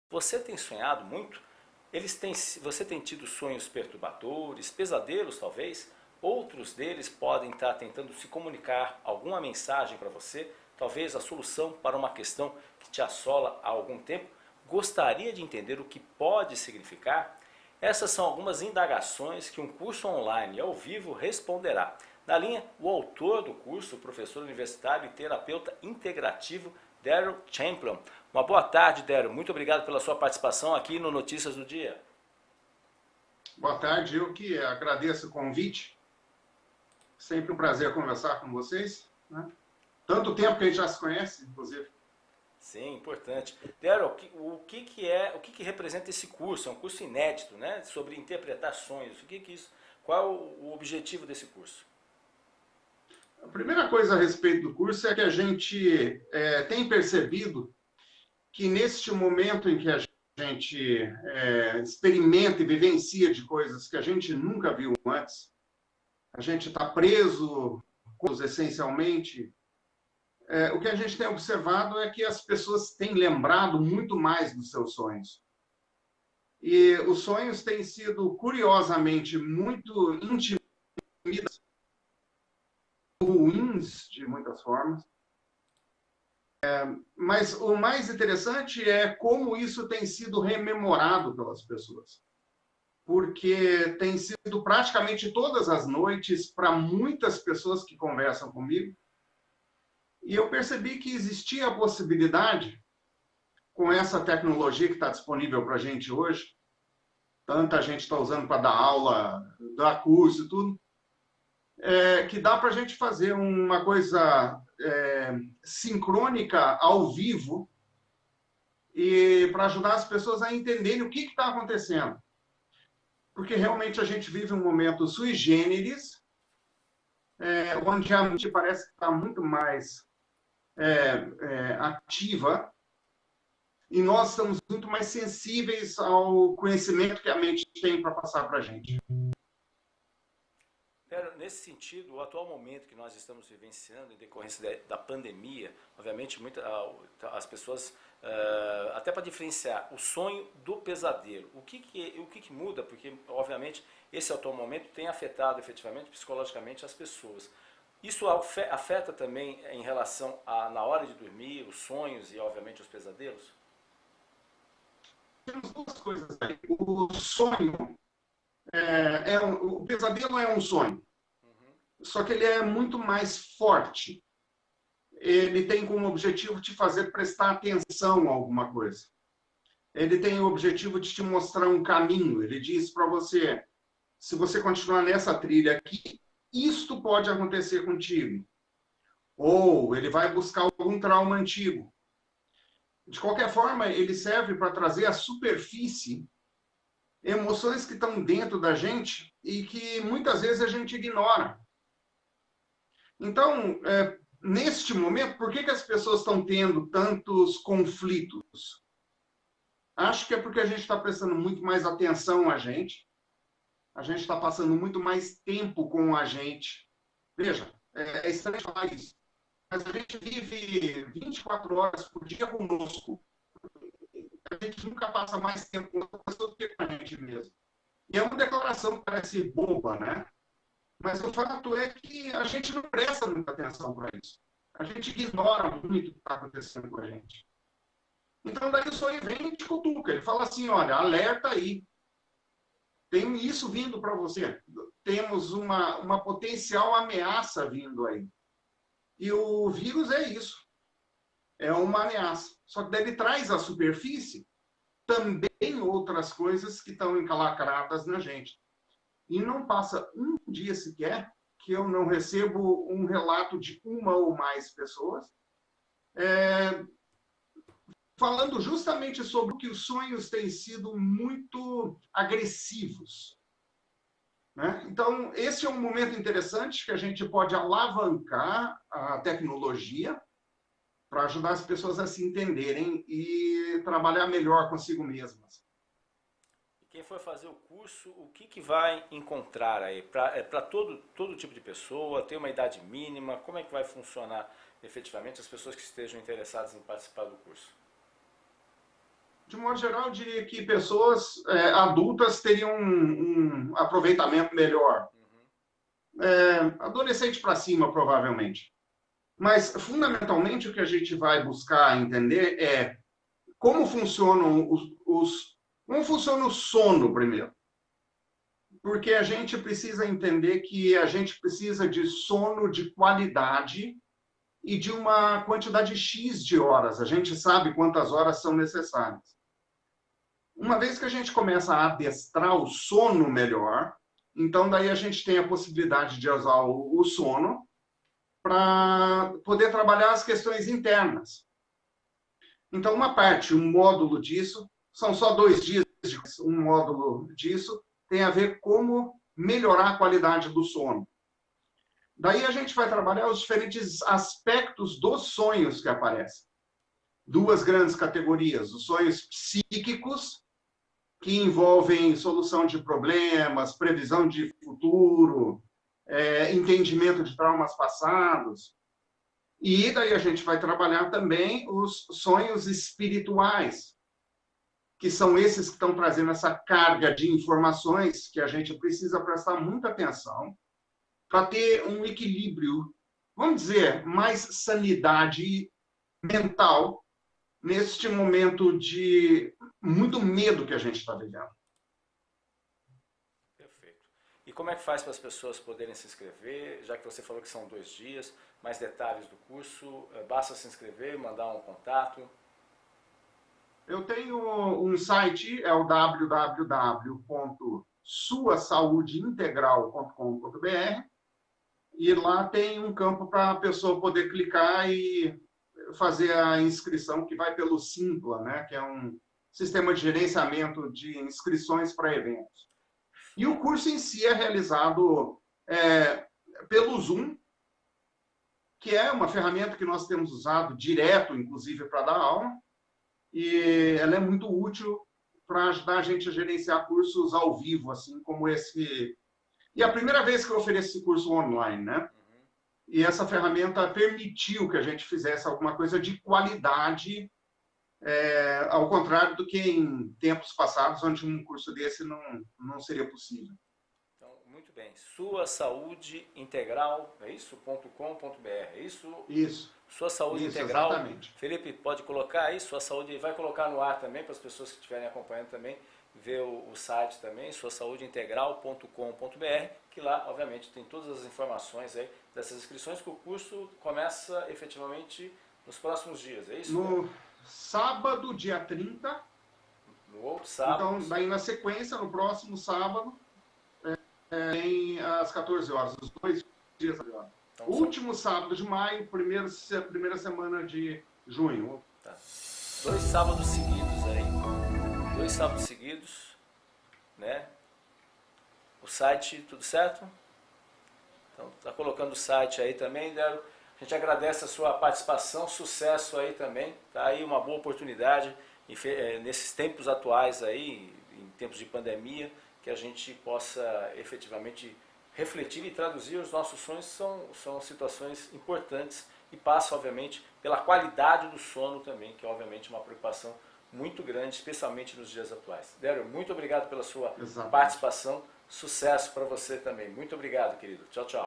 ENTENDA QUAIS SÃO OS SIGNIFICADOS DOS SONHOS
Você tem sonhado muito? Eles têm sido sonhos perturbadores, pesadelos talvez? Outros deles parecem estar tentando comunicar alguma Mensagem, talvez solução para uma questão que te assola há algum tempo? Gostaria de entender o que isso tudo pode significar? Confira a entrevista com o professor universitário e terapeuta integrativo, Darrell Champlin.